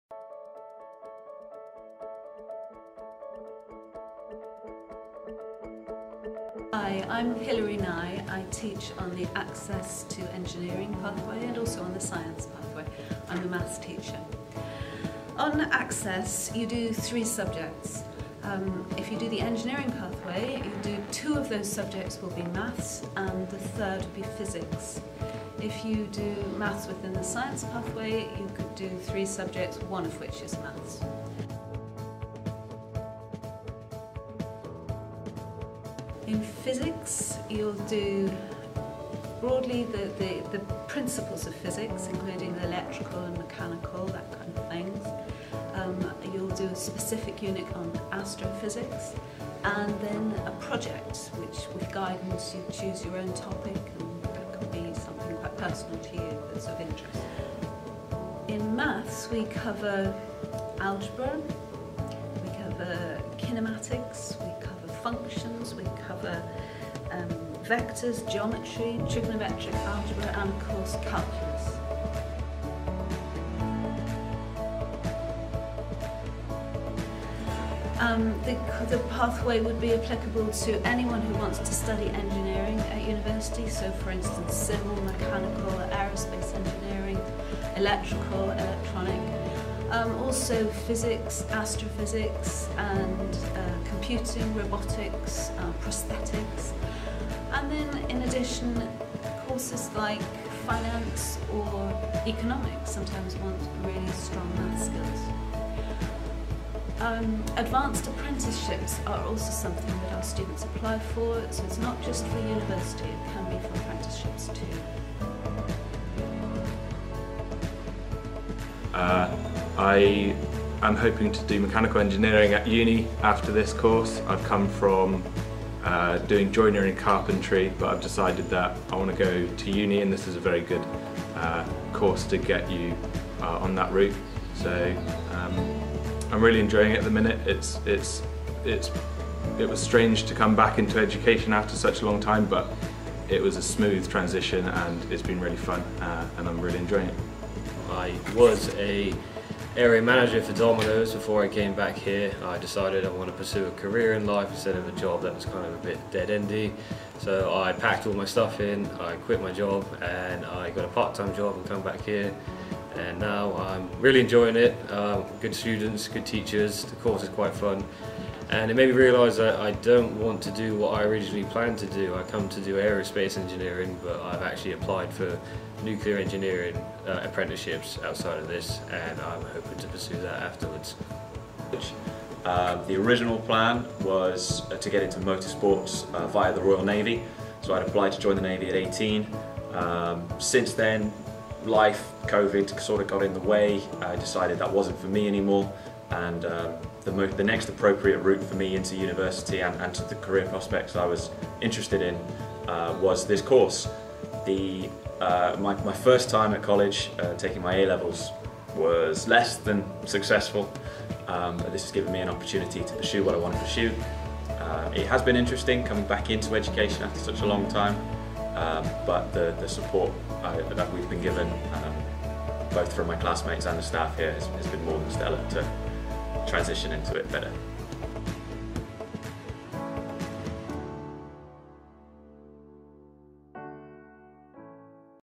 Access to Higher Education - Engineering Science
Find out more about the Access to Higher Education - Engineering Science pathway at City of Bristol College. Discover what you will study, what entry requirements you need and hear from other students on the course.